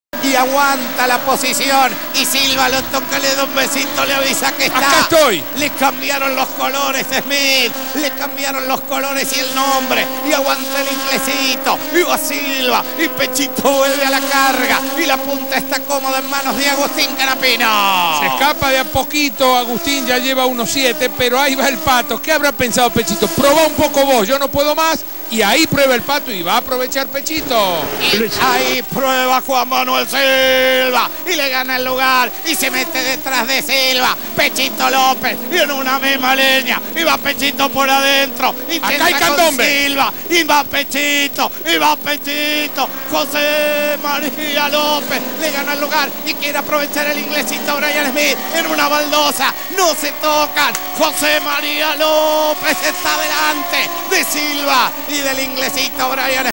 Pechito Lopez en Interlagos 2010
Posicion ganada en el mitico Interlagos de Brasil, sobre el Mondeo del Oil Competicion, a Smith y Silva (TopRace/America24)